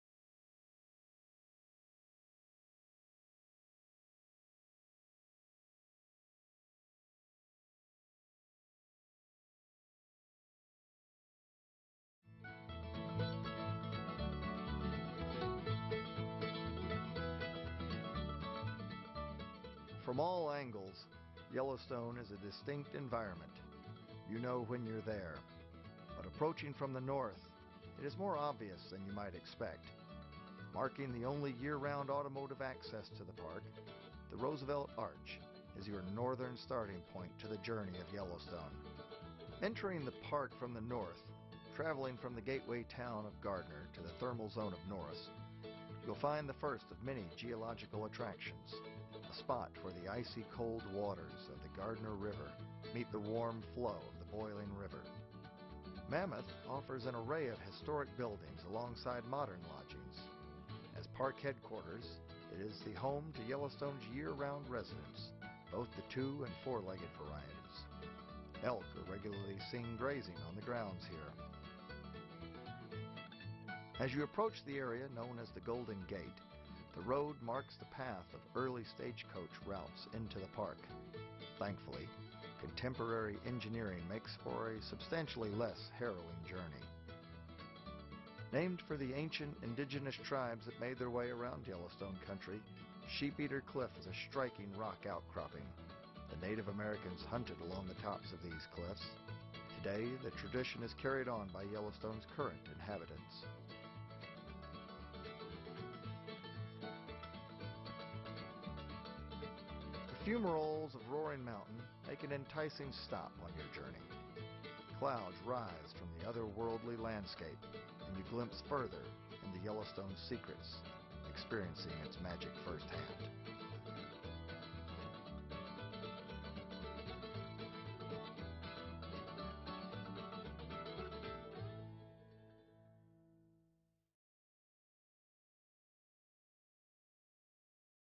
Stati Uniti. Montana. Parco di Yellowstone: da Gardiner a Norris
Il Montana è uno stato degli Stati Uniti, che ospita due bellissimi parchi: il parco di Yellowstone e il parco di Glacier. Il parco di Yellowstone è il parco nazionale più antico del mondo e Patrimonio dell'Umanità che custodice specie animali molto rari come il bisonte americano e l'orso grizzly. Il parco Glacier è un bellissimo parco americano con più di 130 laghi alpini che riflettono nelle acque cristalline il panorama circostante.